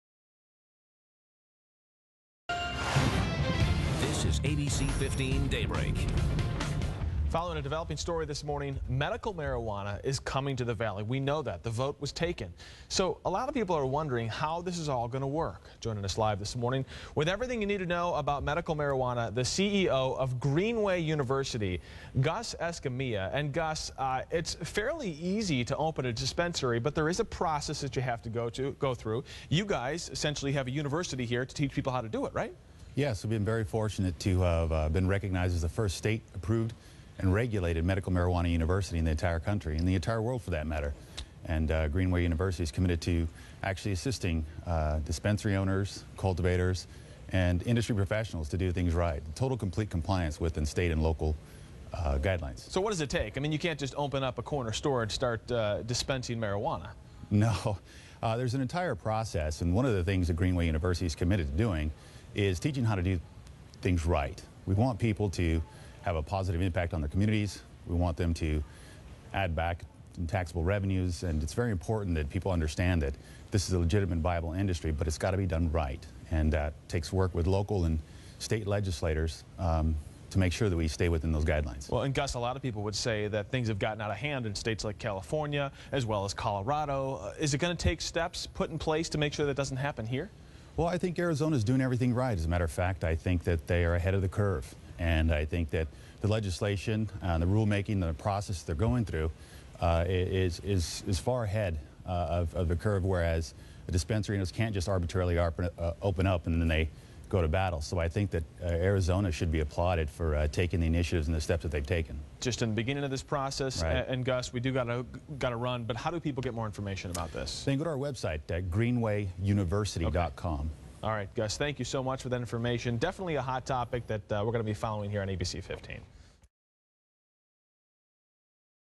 Greenway University CEO and Founder Gus Escamilla on KNXV Arizona
Founder and CEO of Greenway University is interviewed by local television preceding a Medical Marijuana event held in Arizona after voters approved the use of medical marijuana in state.